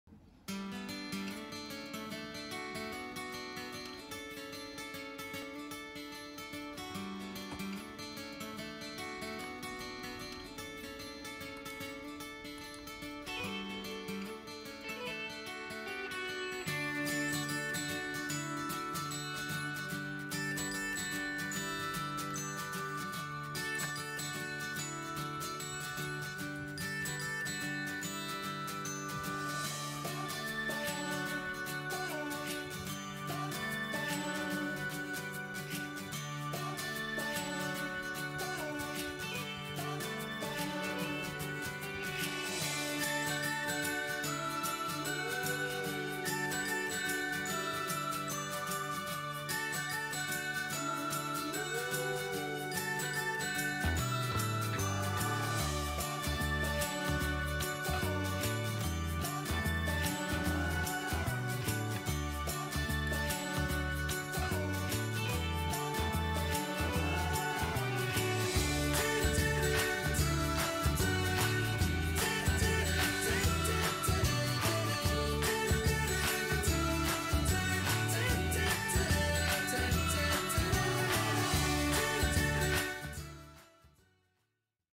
パソコン監視猫Ⅱ
今日もお仕事の監視をしています。

Cat watching work